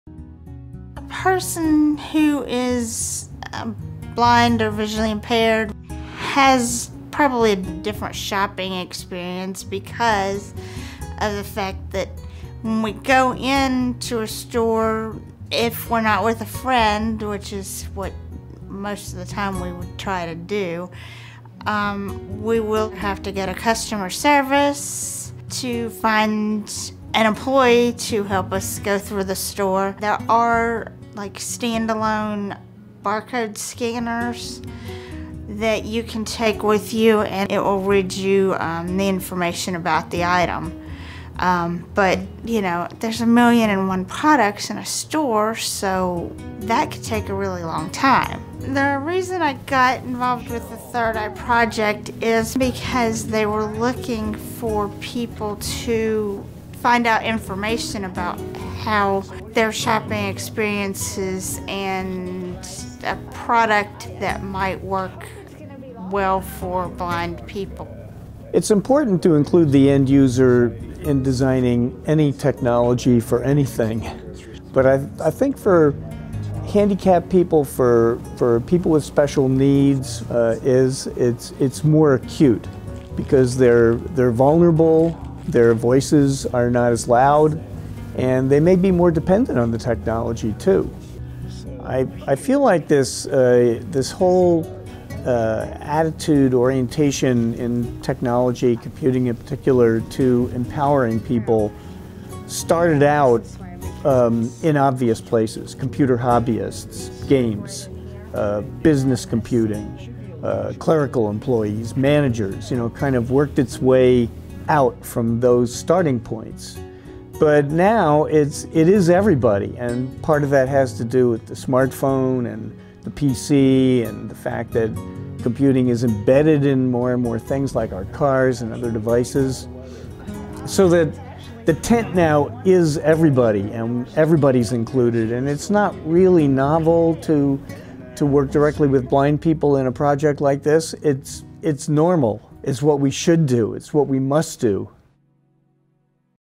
Penn State researcher stresses the need to involve end users during design phase
John Carroll, distinguished professor of Information Sciences and Technology, discusses how he involves the end user in design for the "Third Eye Project"